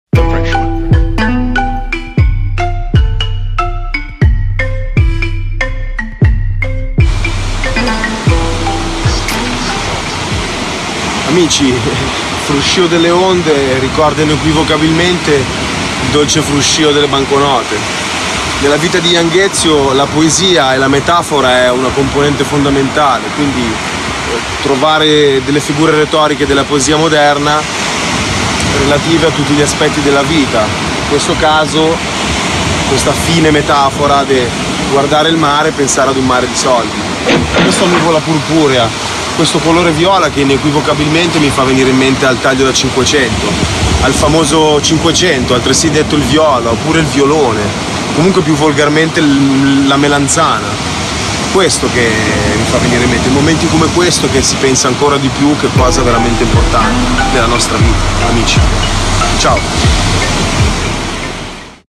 I PREZIOSI CONSIGLI DI YOUNG EZIO - EPISODIO 4
Si dice che la verità ultima delle cose si riesce ad afferrare nella contemplazione della natura. Solo lontano dagli uomini nella purezza degli elementi.
Anche Young Ezio ascoltando il dolce suono delle onde, riesce improvvisamente a capire quali sono veramente le cose che contano nella vita.

Ideato e Diretto da Pablito Maravilla